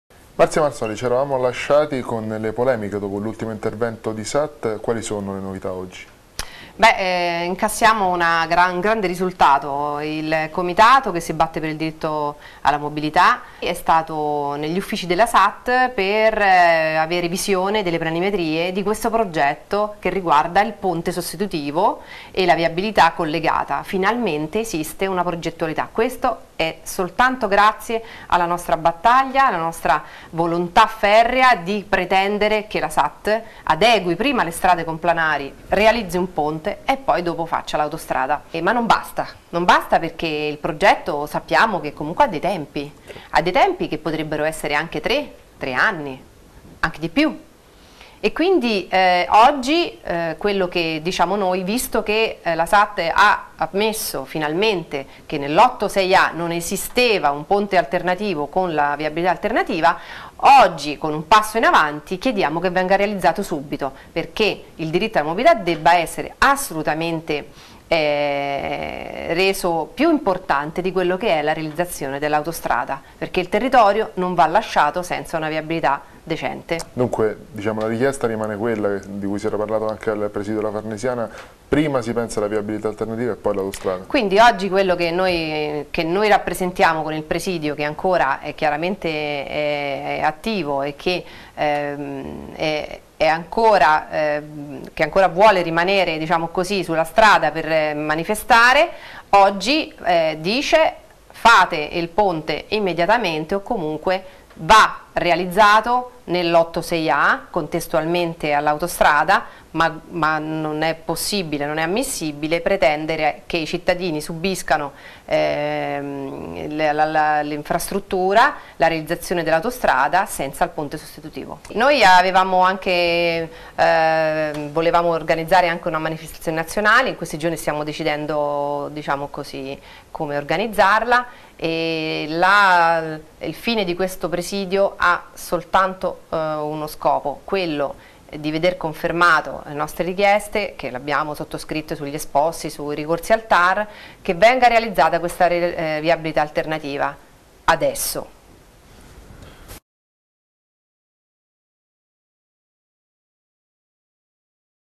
Tirrenica, Marzoli: "Finalmente un progetto per il ponte sul Mignone"
Soddisfazione da parte del Comitato per il Diritto alla Mobilità, che giudica positivamente le ultime novità relative al progetto per la realizzazione del ponte sul Mignone nell'ambito dei lavori per l'Autostrada Tirrenica.